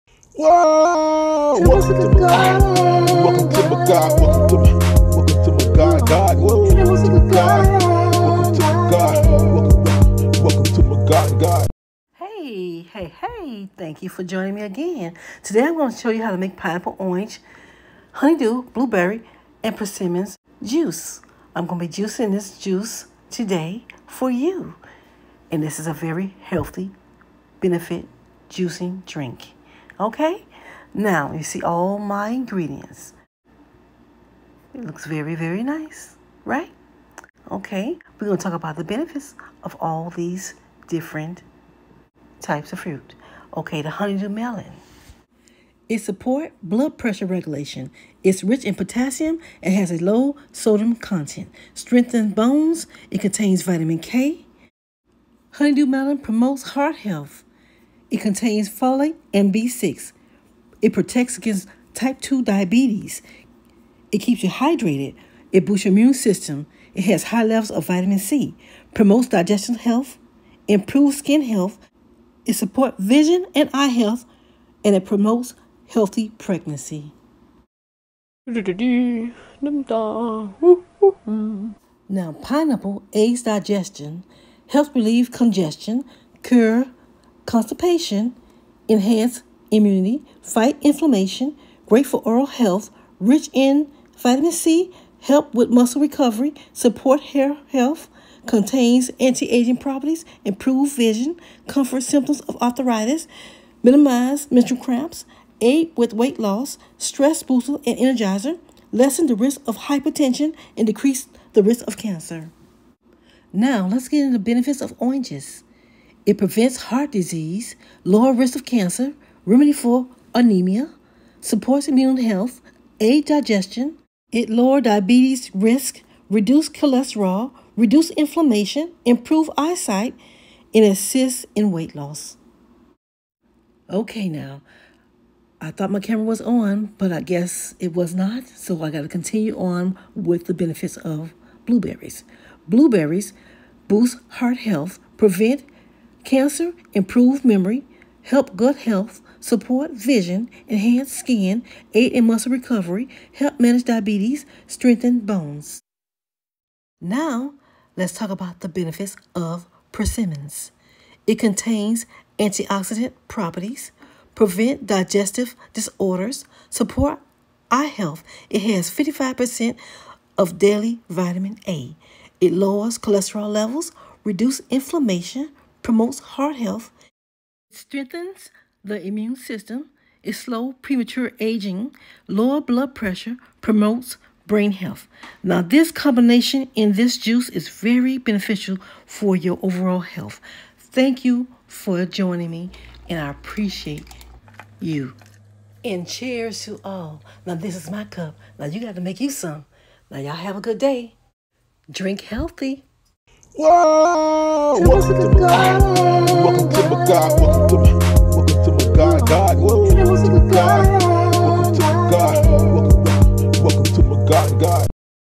Beneficial Superblend of Pineapple, Orange, Honeydew, Blueberries & Persimmon😋💯🔥
Discover the amazing benefits of blending pineapple, orange, honeydew melon, blueberries, and persimmon together for a delicious and nutritious smoothie. Find out how this blend can boost your health and well-being.